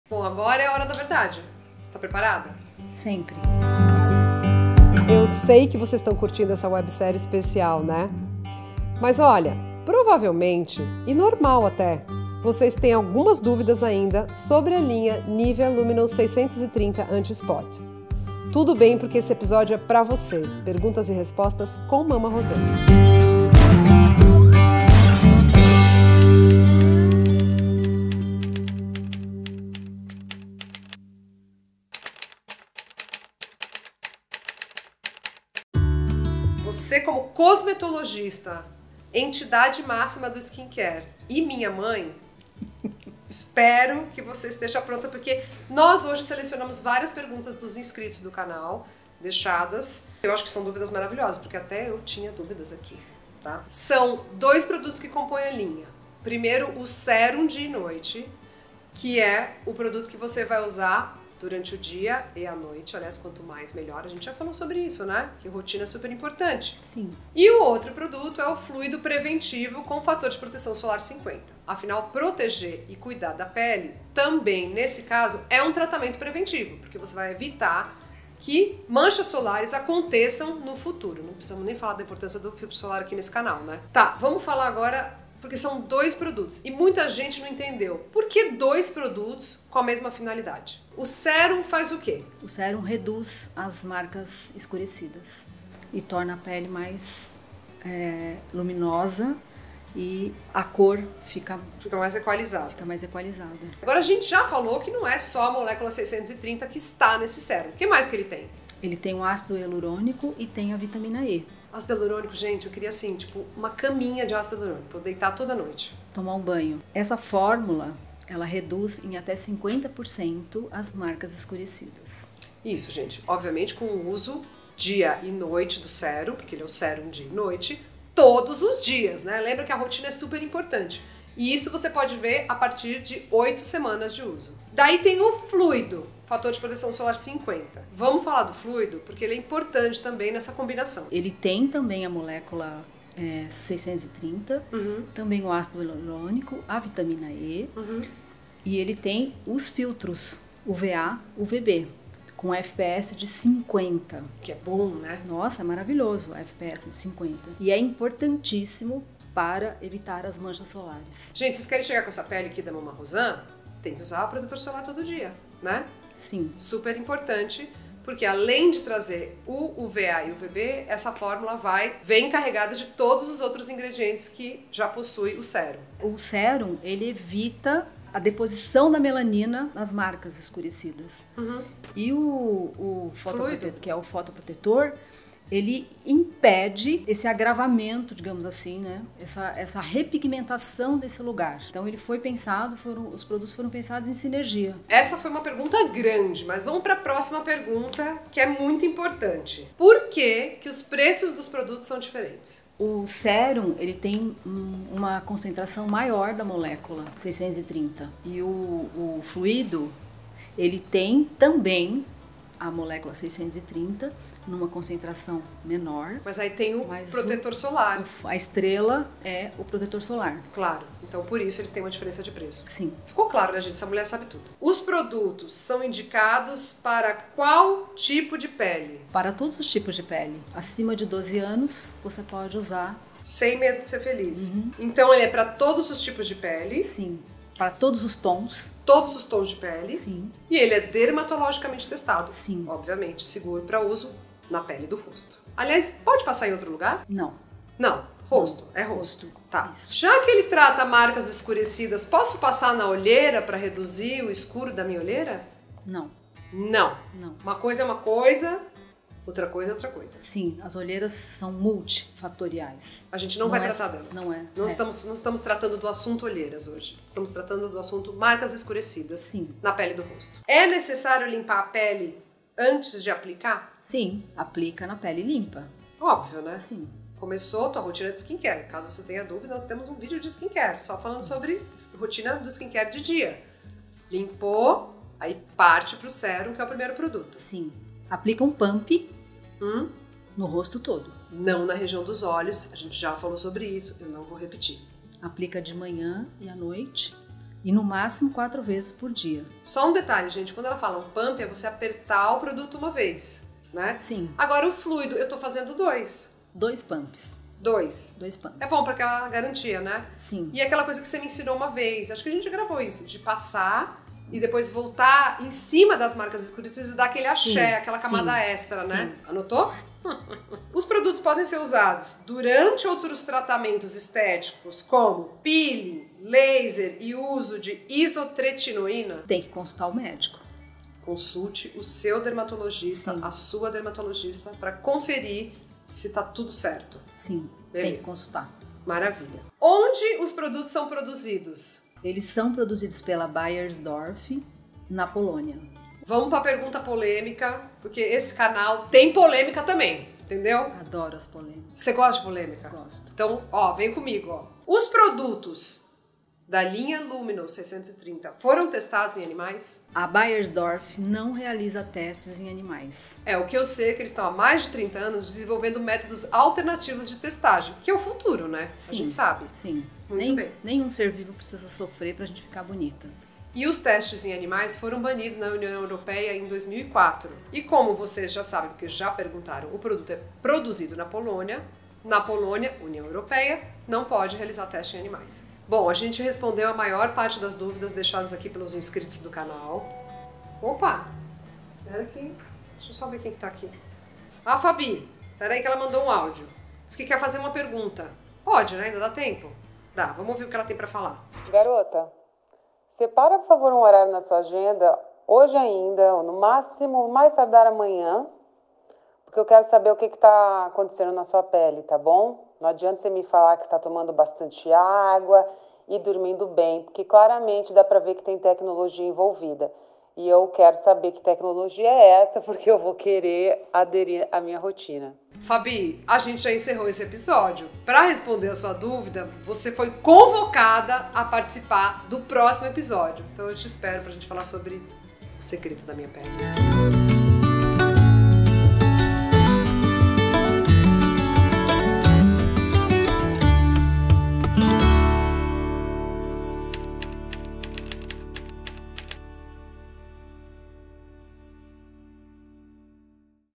ENTENDENDO A LINHA LUMINOUS 630 ANTISPOT [Episódio 04]
Chegamos ao 4º episódio da série e dessa vez chamei a cosmetóloga expert em cuidados Mama Rozan (mais conhecida como minha mãe), para tirar todas as dúvidas que ainda temos sobre a linha 'Luminous 630 Antispot' da Nivea.